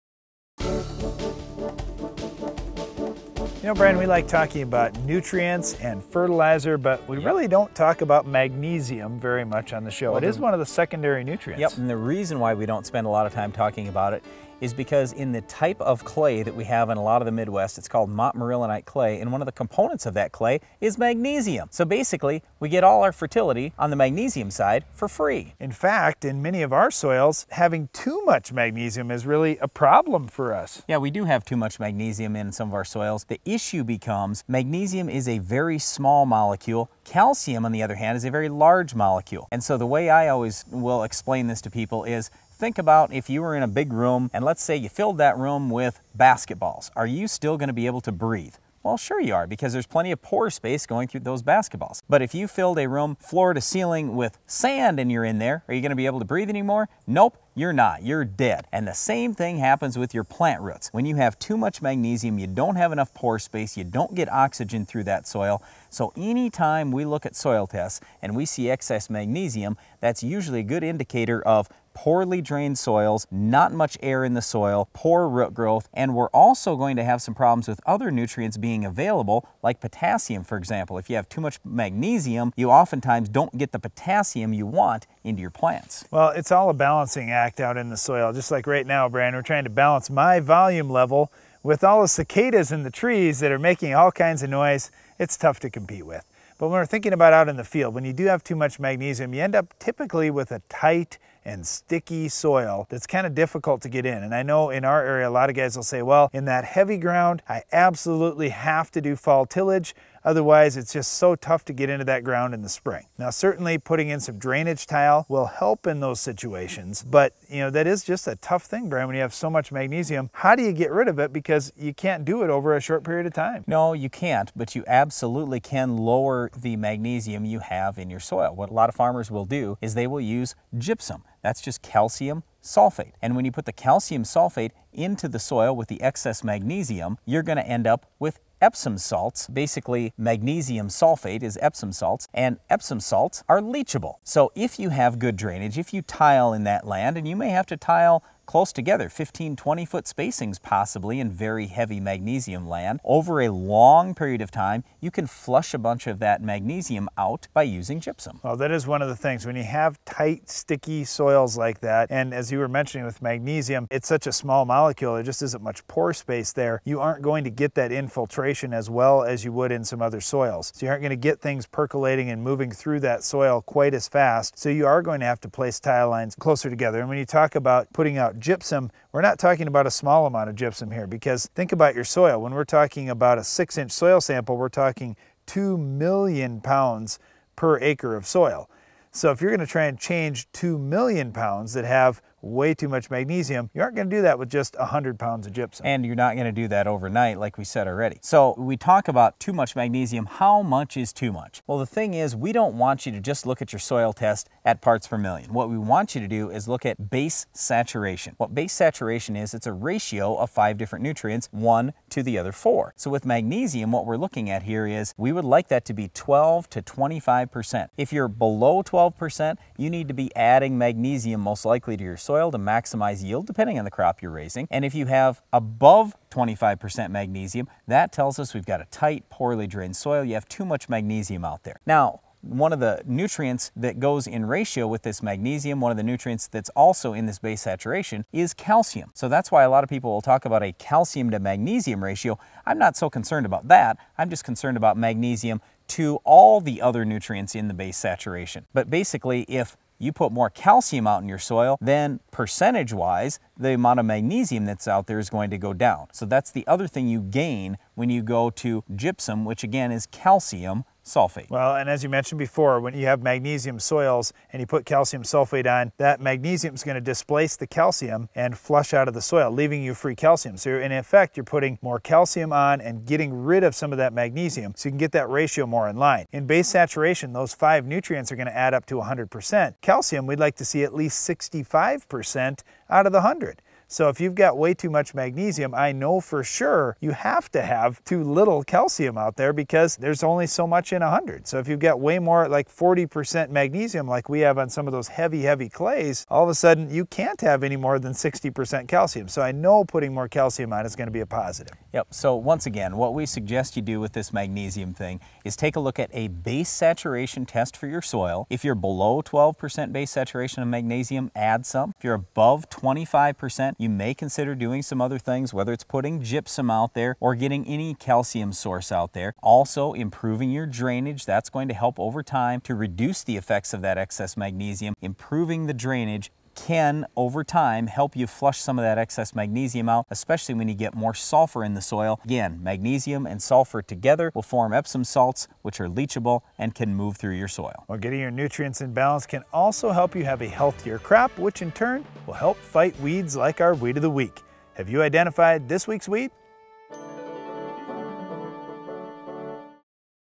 Magnesium #811 (Air Date 10/20/13)
Brian & Darren discuss the importance of magnesium for crops.